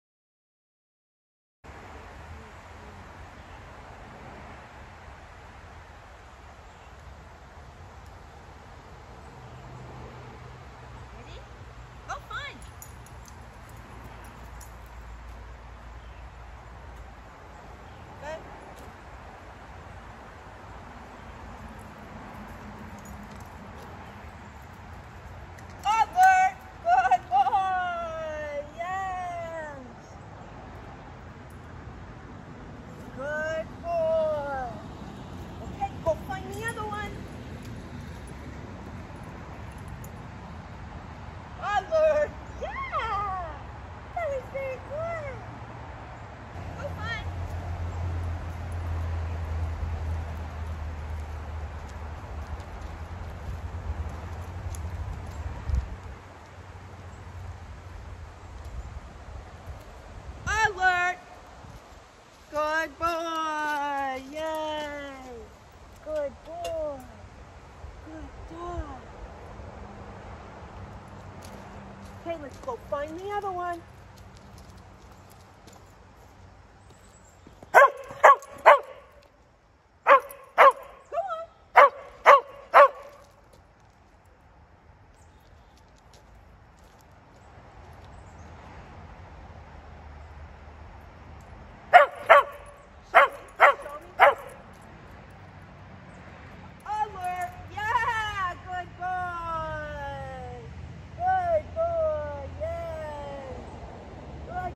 FDSA NW200 Week 3
This video is about multiple hides, 2 scents, on leash, high & low locations